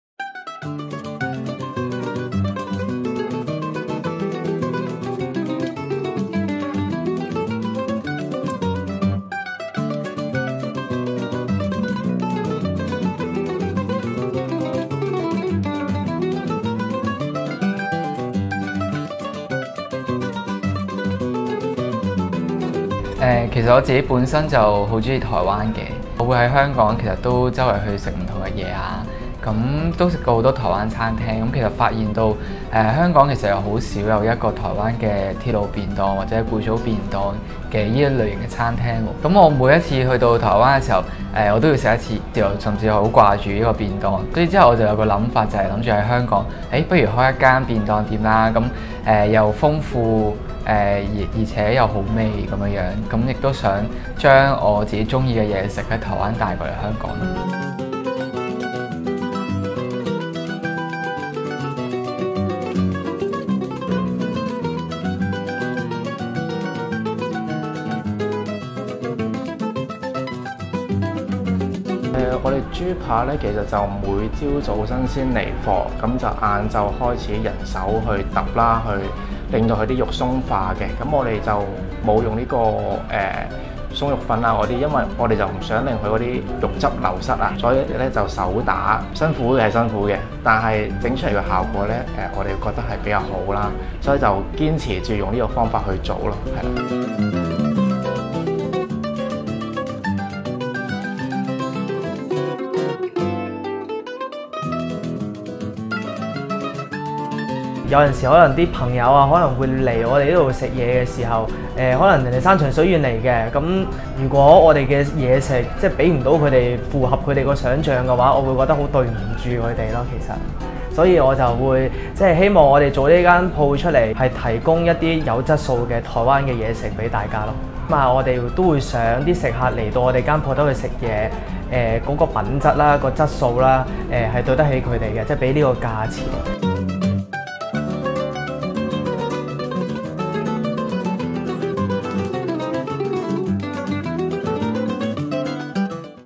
【街坊正店】90 後男孩開文青風台食小店 賣足料滷排骨＋大雞髀便當
【街坊正店】90 後男孩開文青風台食小店
賣足料滷排骨＋大雞髀便當

3個 90 後文青風男孩在大角咀開了間台式便當小店「那個」，主打大大份滷排骨、滷雞腿便當，以及一些從台灣入口的夜市小吃如雞皮卷、芋泥貢丸。他們首次創業想做好出品質素，用好的材料，亦堅持不下化學鬆肉粉，寧願花時間以人手去揼新鮮豬扒使肉質天然鬆化，不愧是良心小店。

【INFO】
那個
地址：大角咀大政街 6-16 號大同新邨大有樓地下 A 號舖
營業時間：12nn-2:30pm，6pm-9:30pm
全方位緊貼《U Magazine》社交平台